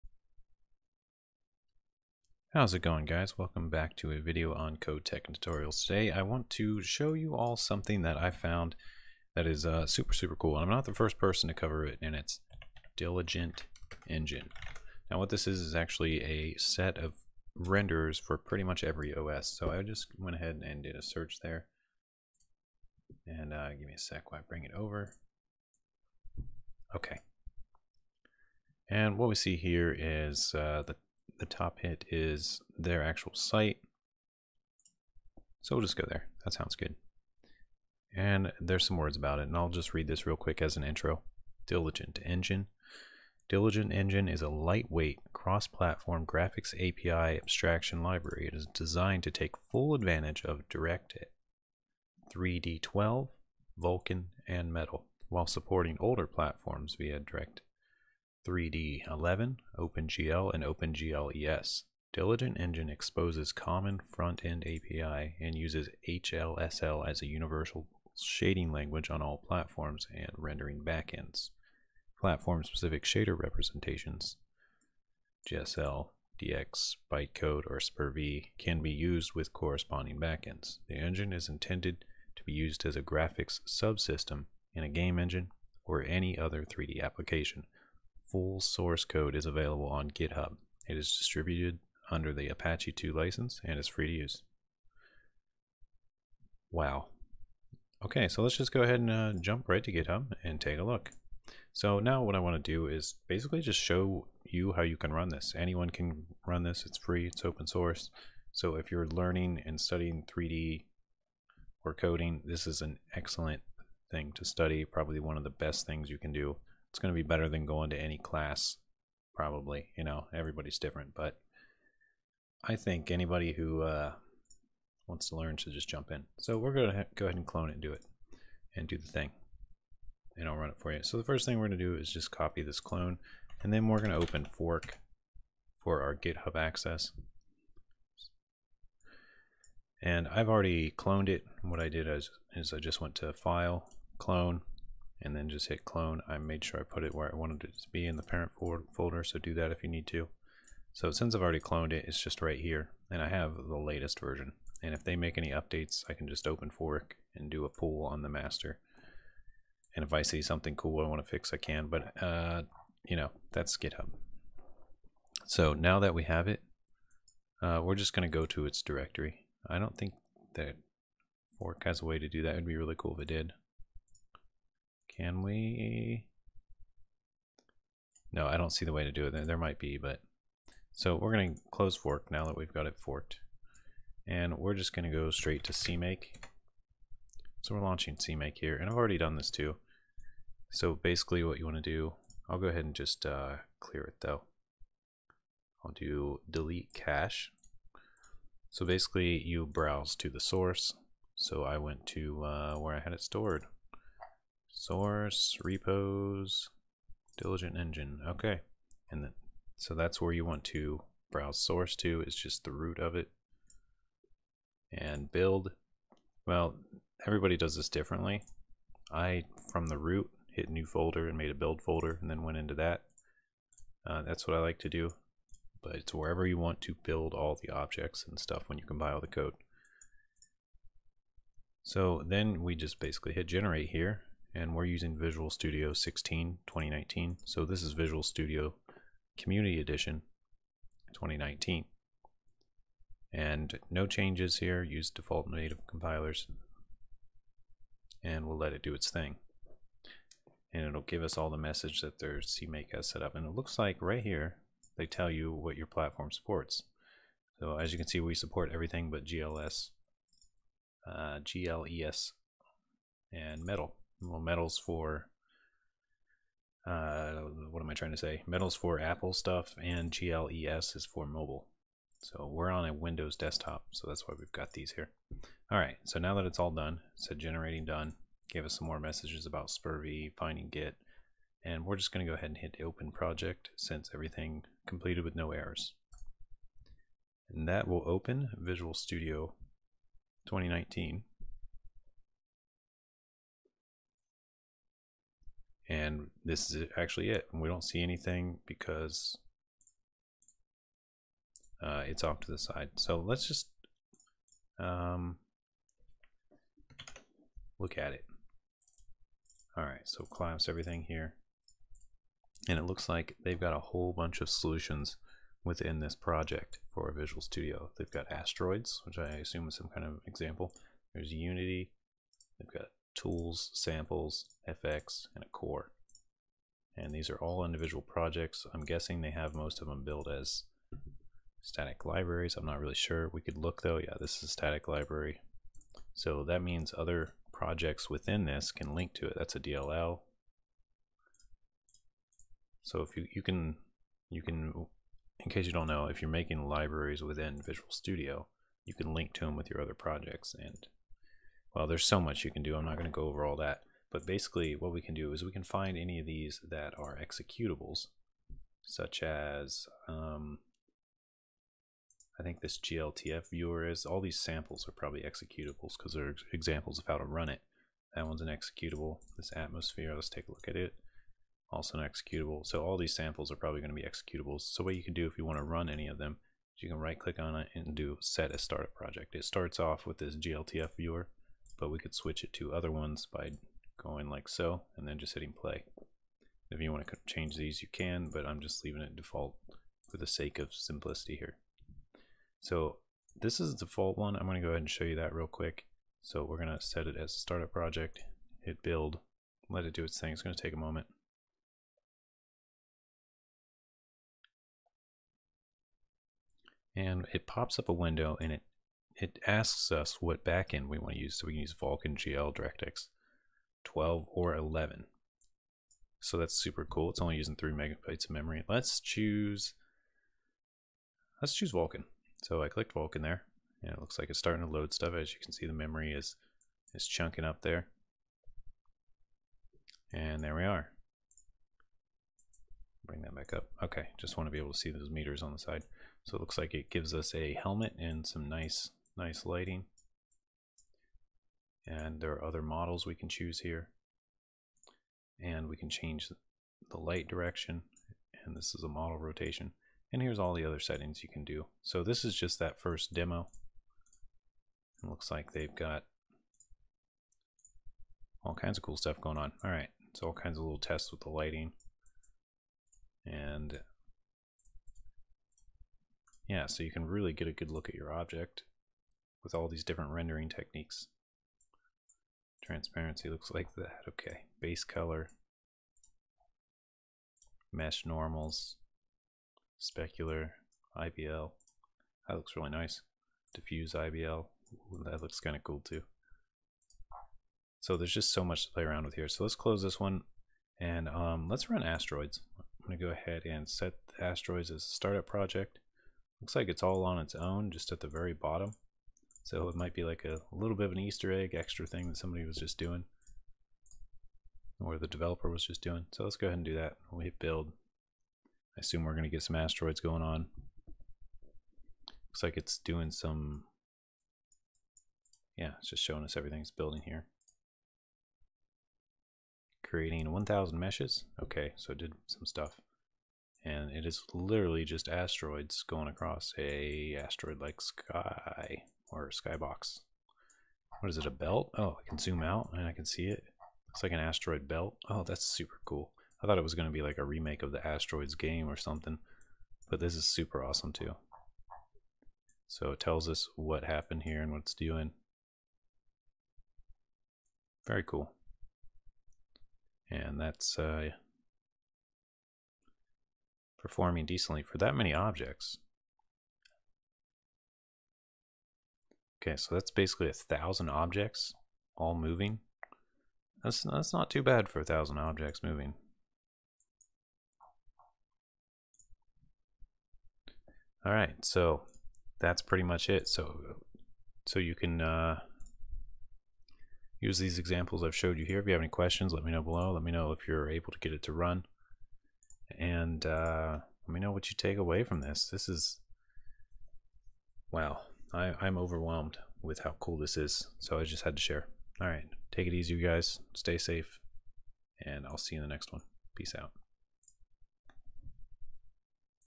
DILIGENT ENGINE - RENDERING DX12/VULKAN/METAL/GL/GLES OMG!
This might be among the coolest open source project I have seen in a while and every coder should probably know about it.  Let me know what you think.

Thanks for coming to the channel to check out a video on Code, Tech, and Tutorials.
Please leave your feedback or questions in the comments!